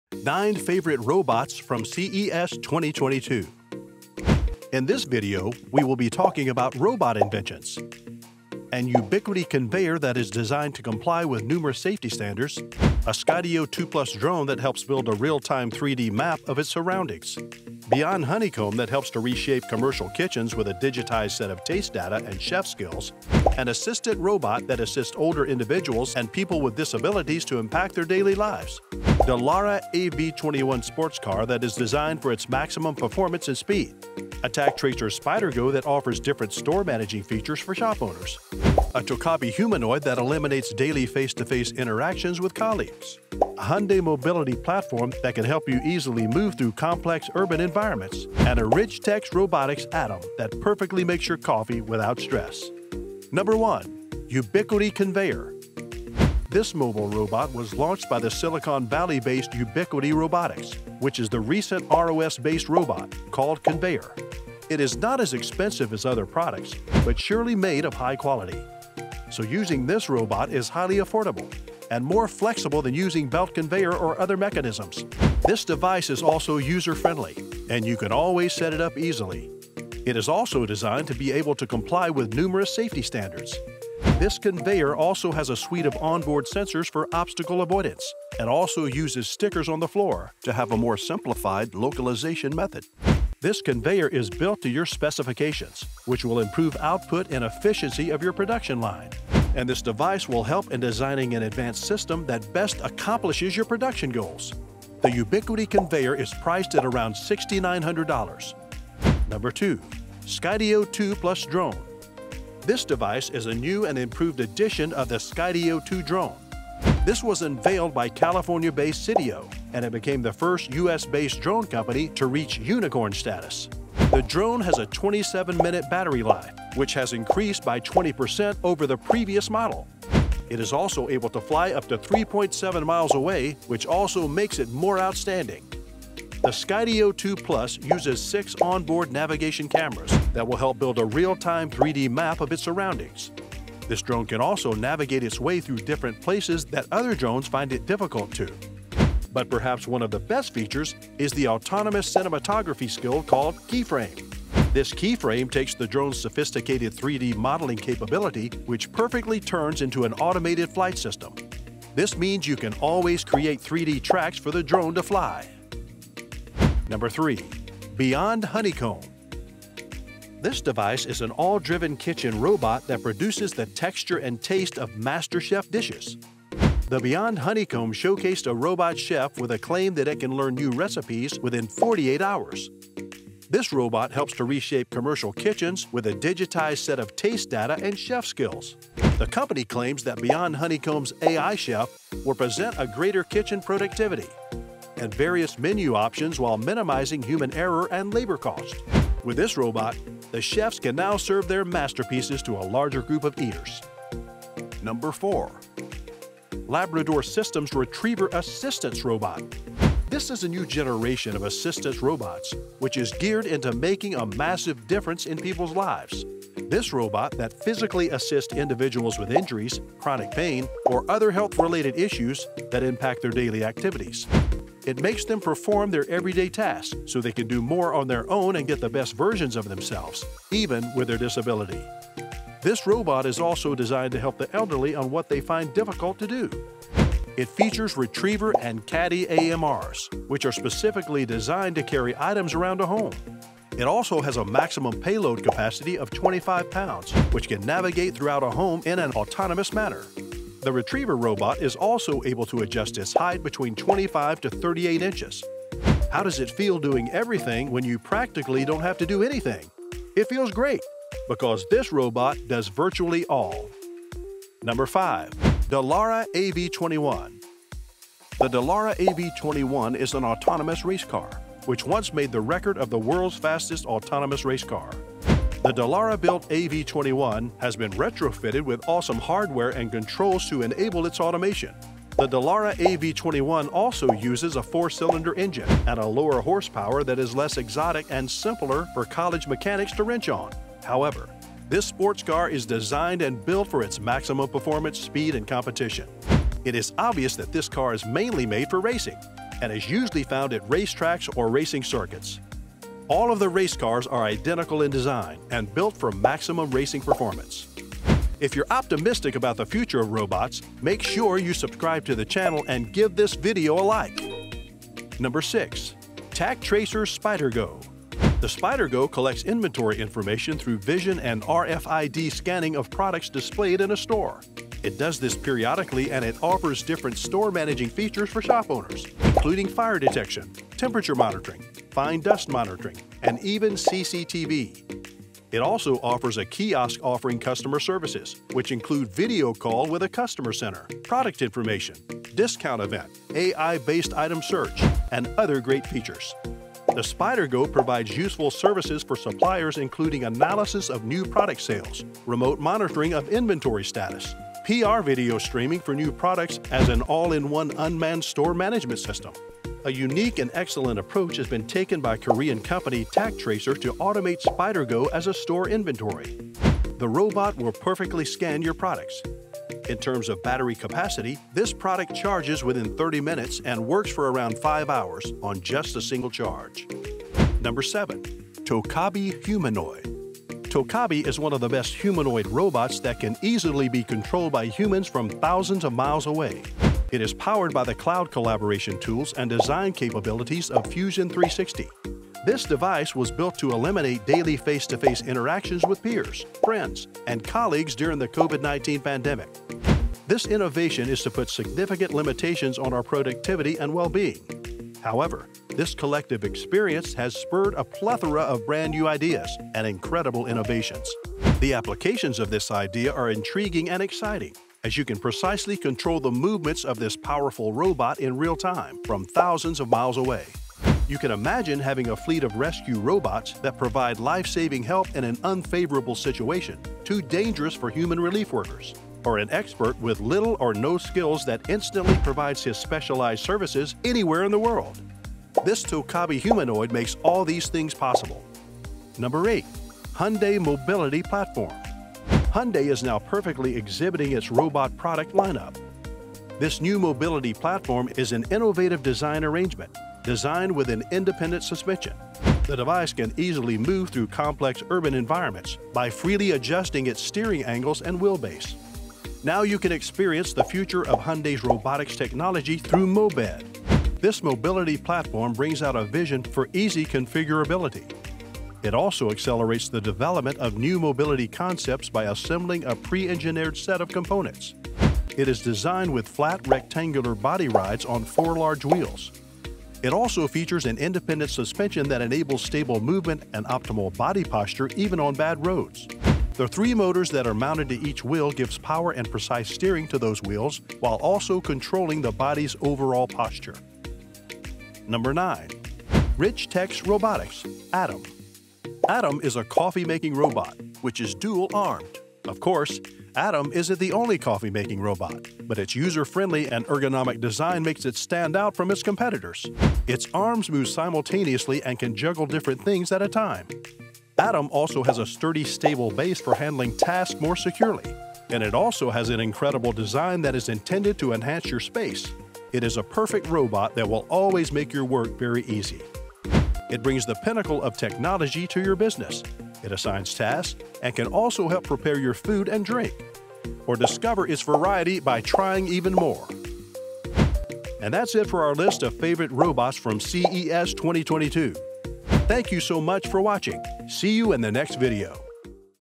Insane Robotic Inventions Of 2022 You Won't Believe exist
Insane Robotic Inventions Of 2022
Timestamps
0:00 Intro
0:53 Ubiquity Conveyor
1:49 Skydio 2+ Drone
2:47 Beyond Honeycomb
3:24 Labrador Systems’ Retriever Assistance Robo
4:24 Dallara AV-21
5:15 Tactracer’s Spider-Go
6:17 TOCABI Umanoid
7:19 Hyundai Mobility Platform
8:15 Richtech Robotics’ ADAM
8:58 Outro

Ubiquity Conveyor: This mobile robot was launched by the Silicon Valley-based Ubiquity Robotics, which is the recent ROS-based robot, called Conveyor. It is not as expensive as other products but surely made of high quality. So, using this robot is highly affordable and more flexible than using belt conveyance or other mechanisms. This device is also user-friendly, as you can always set it up easily. It is also designed to be able to comply with numerous safety standards.
Skydio 2+ Drone: This device is a new and improved edition of the Skydio 2 drone. This was unveiled by California-based Sydio, and it became the first U.S.-based drone company to reach unicorn status. The drone has a 27-minute battery life, which has increased by 20% over the previous model. It is also able to fly up to 3.7 miles away, which also makes it more outstanding. The Skydio 2+ uses six onboard navigation cameras that will help build a real-time 3D map of its surroundings.

Beyond Honeycomb: This device is an all-driven kitchen robot that produces the texture and taste of master chef dishes. The Beyond Honeycomb showcased a robot chef with the claim that it can learn new recipes within 48 hours. This robot helps to reshape commercial kitchens with a digitized set of taste data and chef skills.

Labrador Systems’ Retriever Assistance Robot: This is a new generation of assistance robots, which is geared into making a massive difference in people’s lives. This robot that physically assists individuals with injury, chronic pain, or other health-related issues that impact their daily activities. It makes them perform their everyday tasks, so they can do more on their own and get the best version of themselves, even with their disability.

Dallara AV-21: The Dallara AV-21 is an autonomous racecar, which once made the record of the world’s fastest autonomous racecar. The Dallara-built AV-21 has been retrofitted with awesome hardware and controls to enable its automation. The Dallara AV-21 also uses a four-cylinder engine and a lower horsepower that is less-exotic and simpler for college mechanics to wrench on.

Tactracer’s Spider-Go: The SPIDER-GO collects inventory information through vision and RFID scanning of products displayed in a store. It does this periodically, and it offers different store managing features for shop owners, including fire detection, temperature monitoring, fine dust monitoring, and even CCTV. It also offers a Kiosk offering customer services, which include video call with a customer center, product information, discount event, AI-based item search, and other great features.

TOCABI Umanoid: TOCABI is one of the best humanoid robots that can easily be controlled by humans from thousands of miles away. It is powered by the cloud collaboration tools and design capabilities of Fusion 360. This device was built to eliminate daily face-to-face interactions with peers, friends, and colleagues during the COVID-19 pandemic.

Hyundai Mobility Platform: Hyundai is now perfectly exhibiting its robot product lineup. This new mobility platform is an innovative design arrangement, designed with an independent suspension. The device can easily move through complex urban environments by freely adjusting its steering angles and wheelbase. Now you can experience the future of Hyundai’s robotics technology through MobED!

Richtech Robotics’ ADAM: ADAM is a coffee making robot, which is dual-armed. Of course, ADAM isn’t the only coffee-making robot, but its user-friendly and ergonomic design makes it stand out from its competitors. Its arms move simultaneously and can juggle different things at a time. ADAM also has a sturdy, stable base for handling tasks more securely, and it also has an incredible design that is intended to enhance your space.